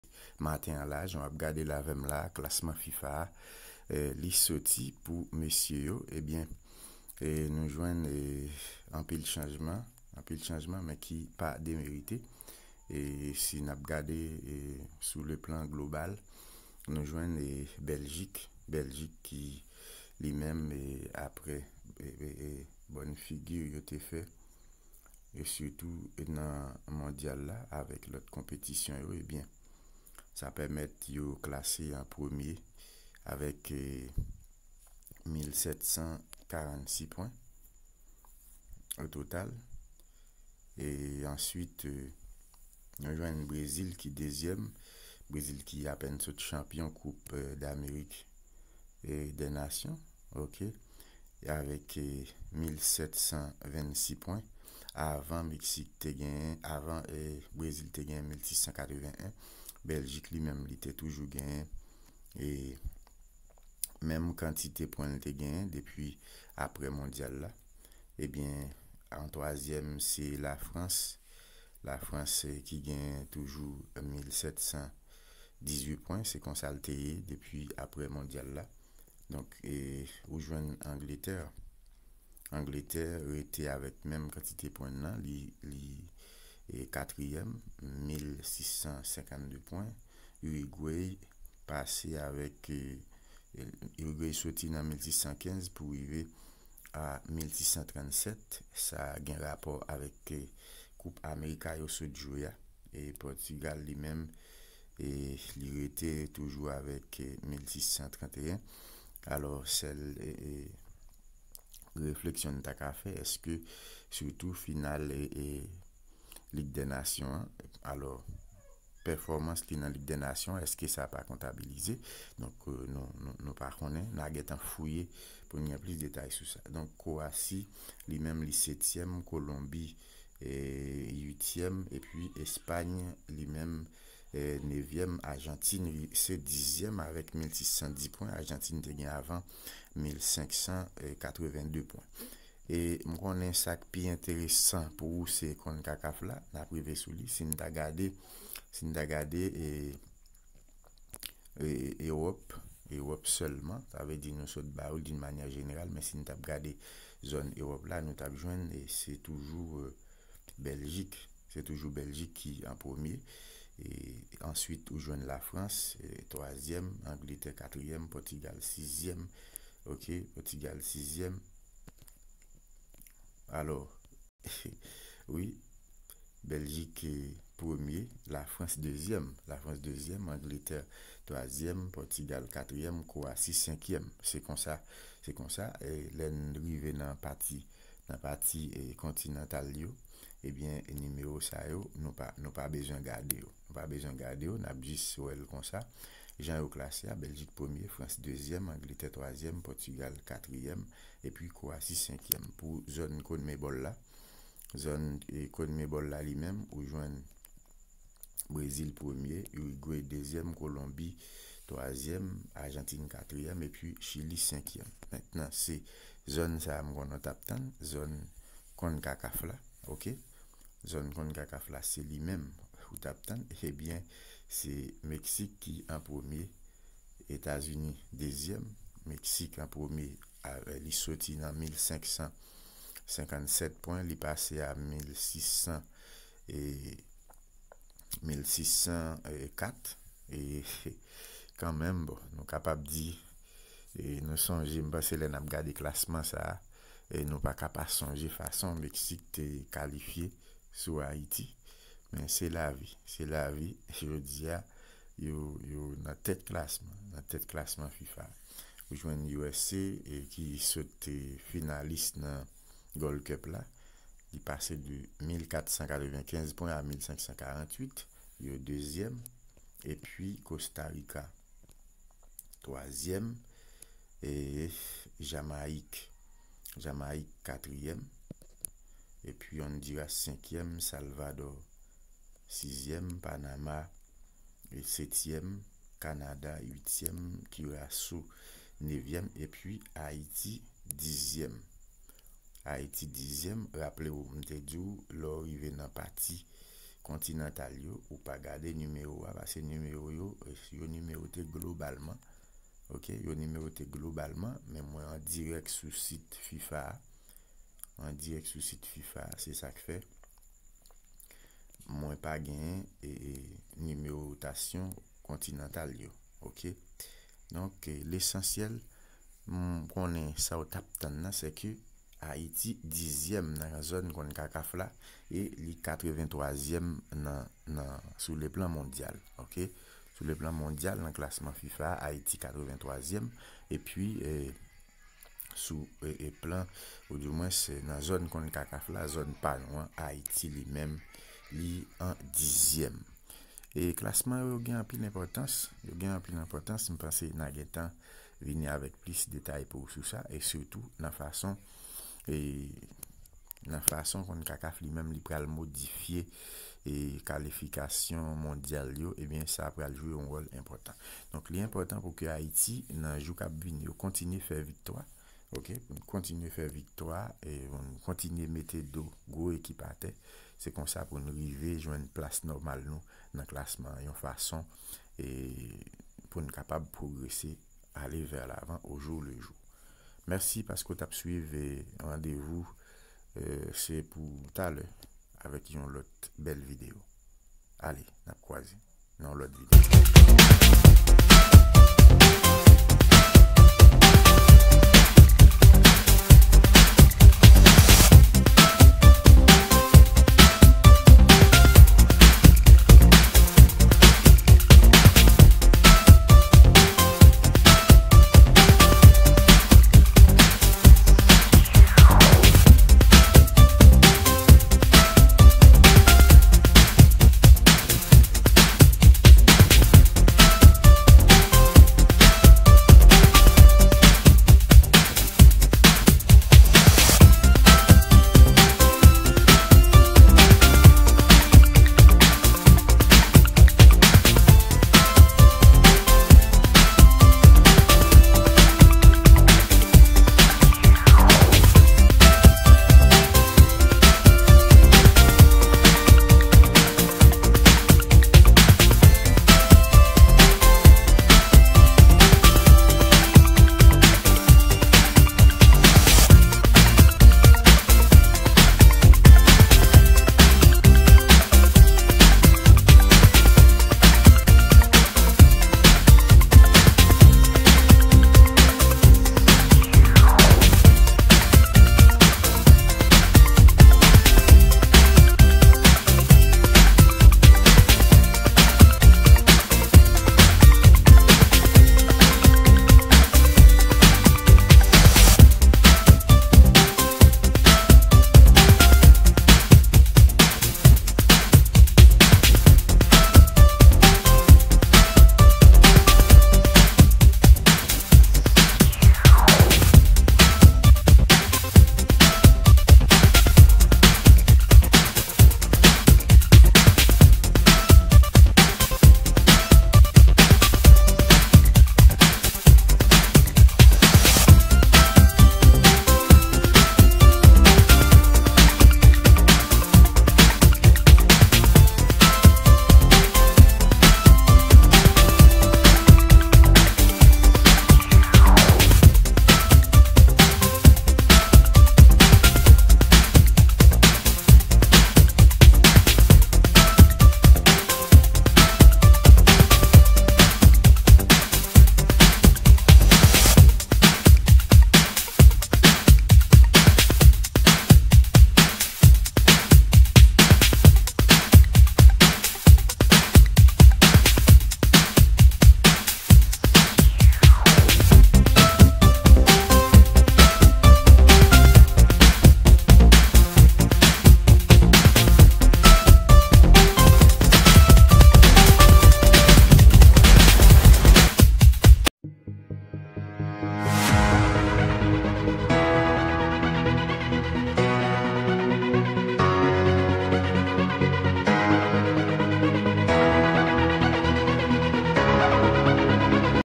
Le classement MONDIAL FIFA 25 juillet 2019
Le classement mondial FIFA Juillet 2019
18 Place Sélection Haitienne de football la janbe gras ak bel pèfomans grenadye yo nan Goldcup la.